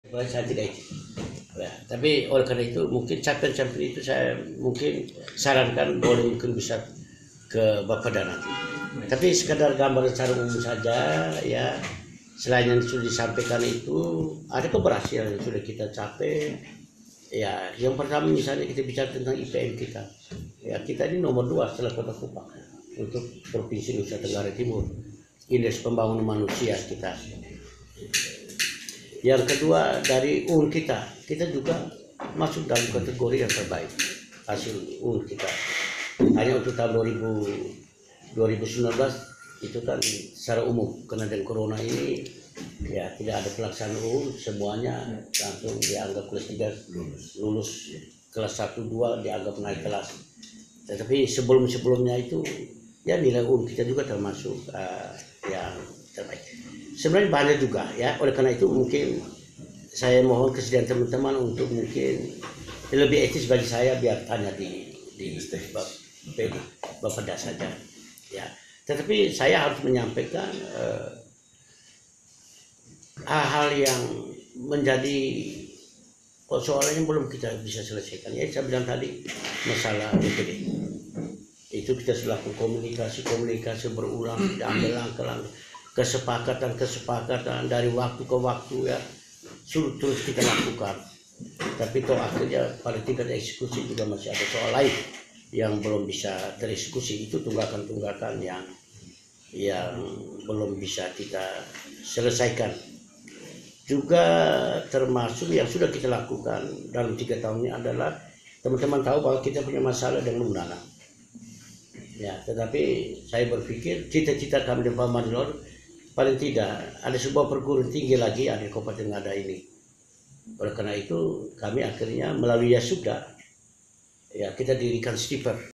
Prestasi Bupati Ngada, Soliwoa, Berikut Video Lengkapnya
Ada sejumlah prestasi yang diraih oleh Paulus Soliwoa.